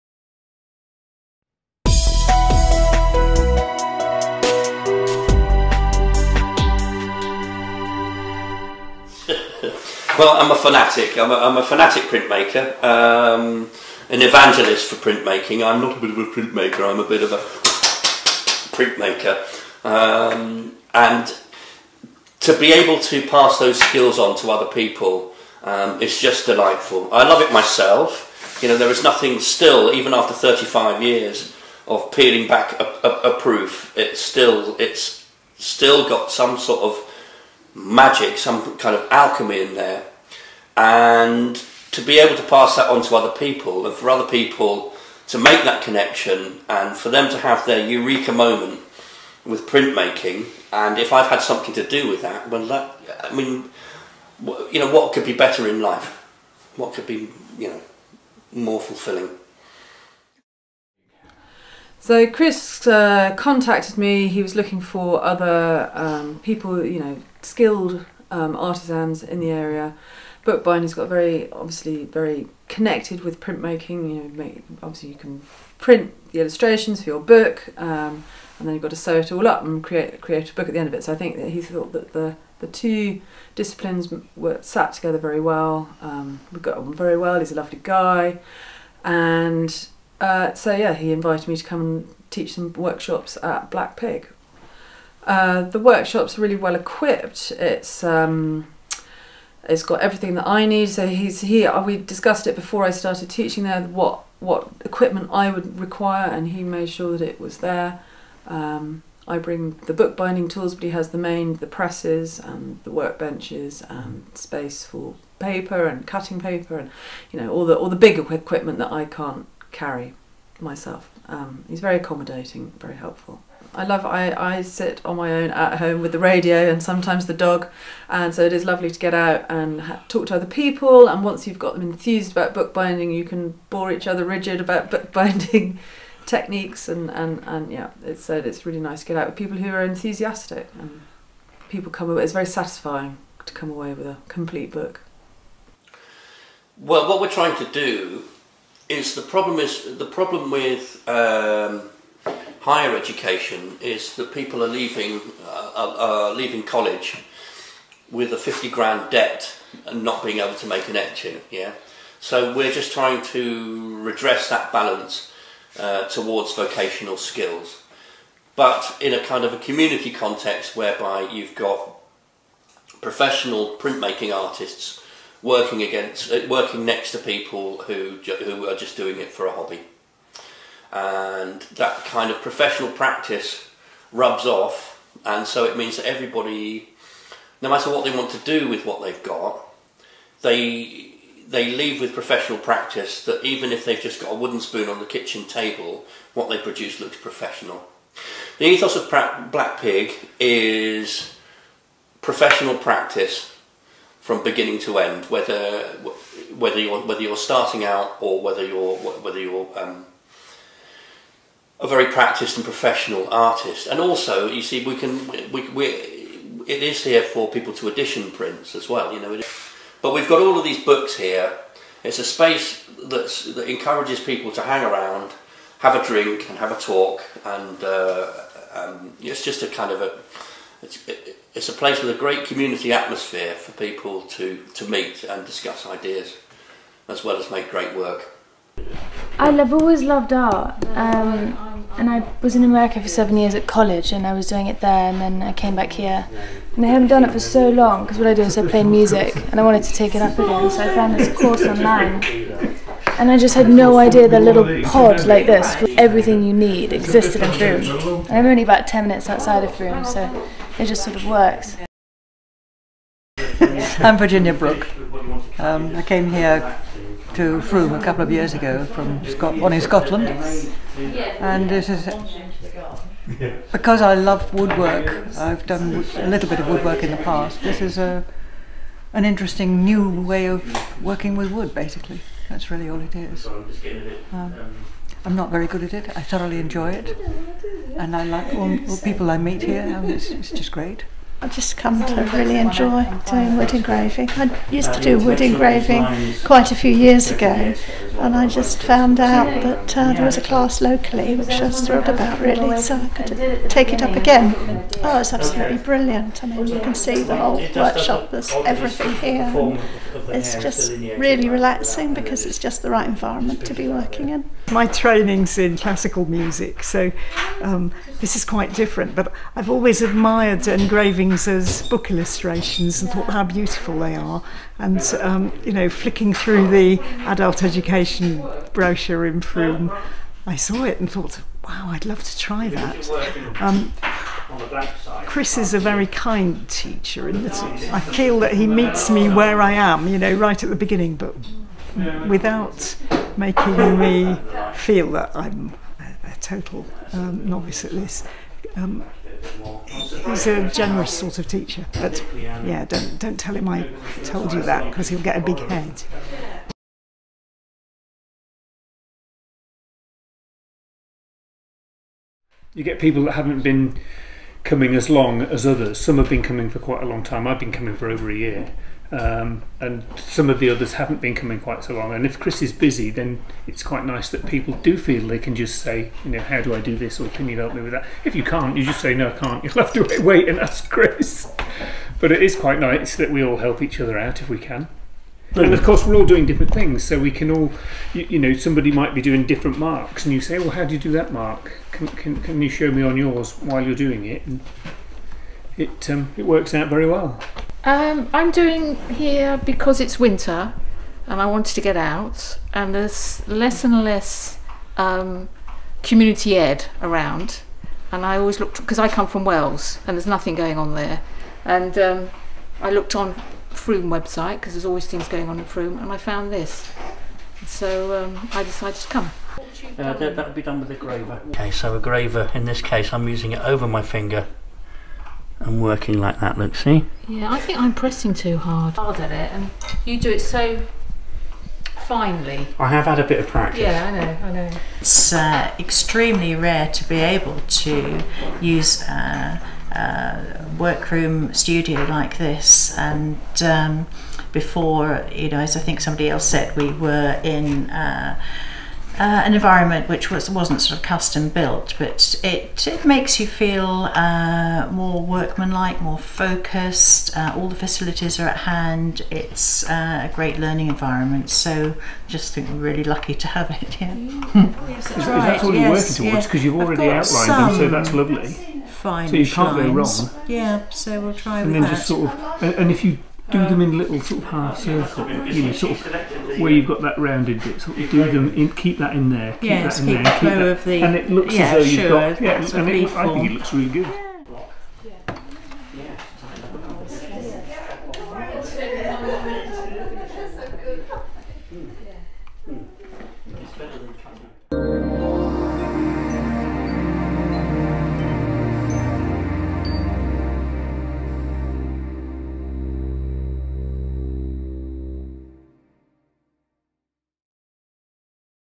Black Pig Print Making
A guided tour of Chris Pig's Black Pig studio and workshop including interviews with some of the printers and artists,